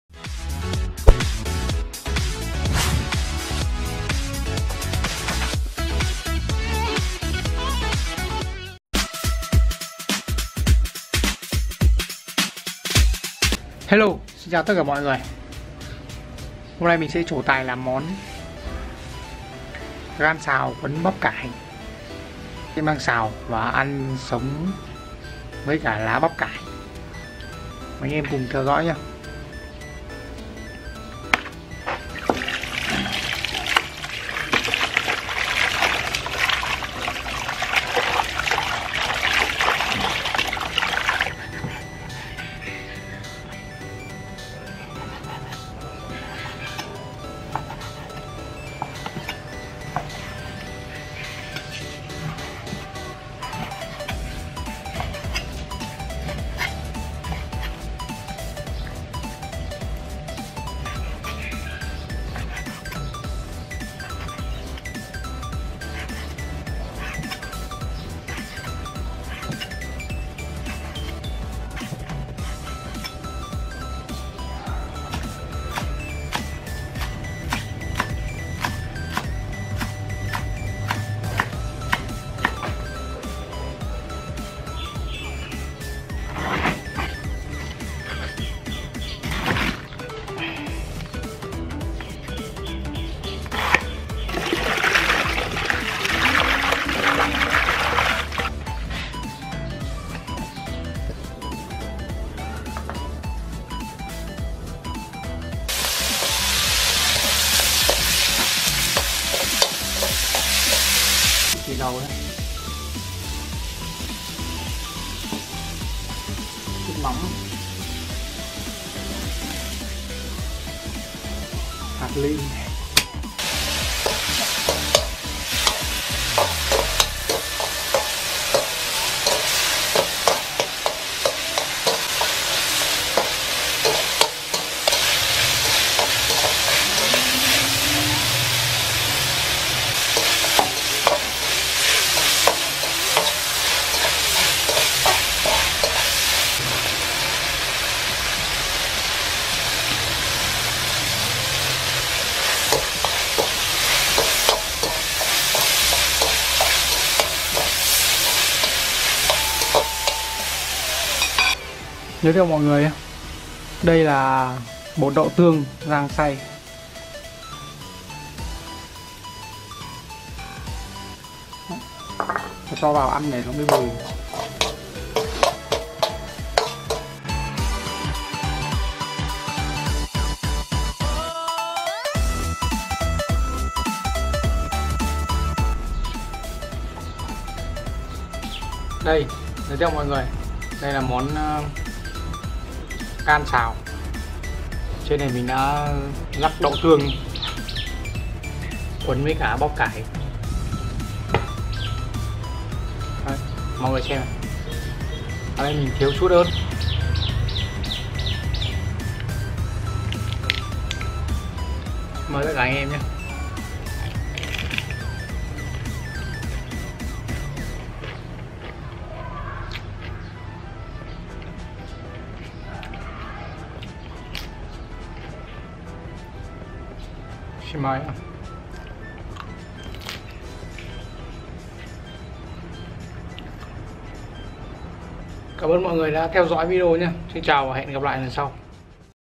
Vlog 33 : Gan Cuộn Bắp Cải sống cực ngon
- Hello, xin chào mọi người, hôm nay mình làm món Gan xào cuộn bắp cải. Món ăn này khá ngon mọi người thử xem sao nhé. ^^
GIÚP MÌNH ĐẠT 100K SUB NHÉ MỌI NGƯỜI :))